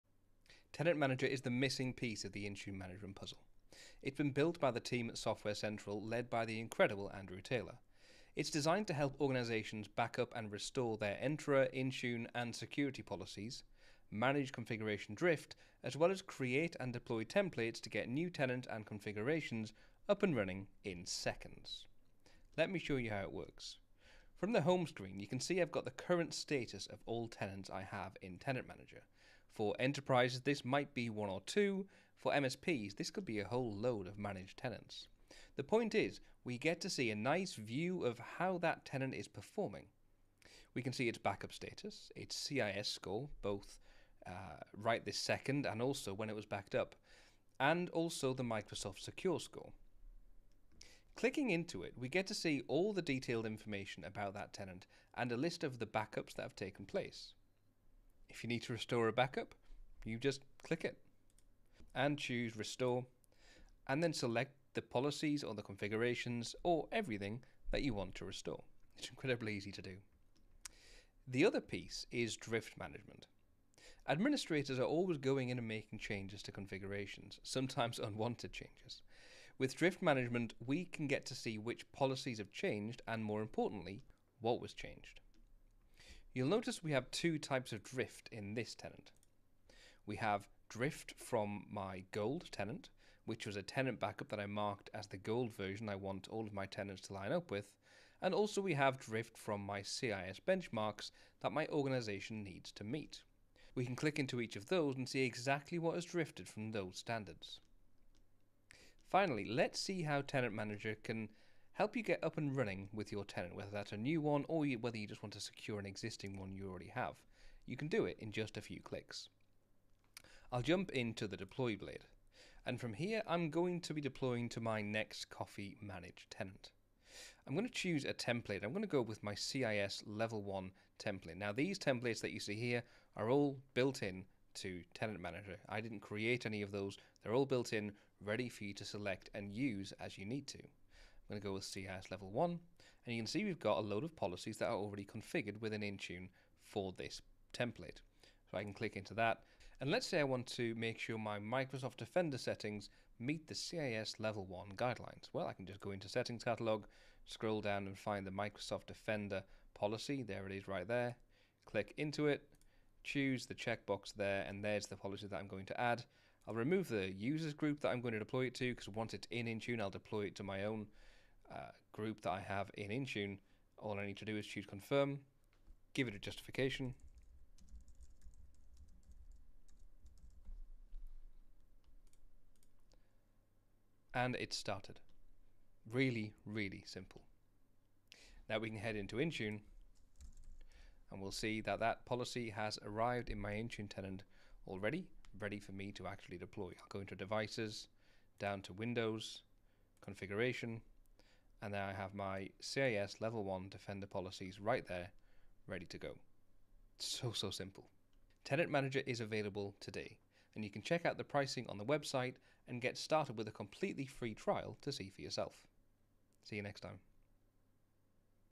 Introducing Tenant Manager - Powerful Backup, Restore and Drift Management for Intune and Entra ID!
Tenant Manager is the missing piece of the Intune management puzzle, developed by SoftwareCentral and led by Andrew Taylor. In this video, we walk through how it helps organisations manage Entra, Intune, and security policies with ease.

Monitor backup status, CIS benchmarks, and Secure Scores across tenants
Restore from backup in seconds
Track configuration drift and see exactly what changed
Deploy secure, CIS-aligned templates to new tenants in just a few clicks

Ideal for enterprises and MSPs alike, Tenant Manager gives you control, visibility, and peace of mind.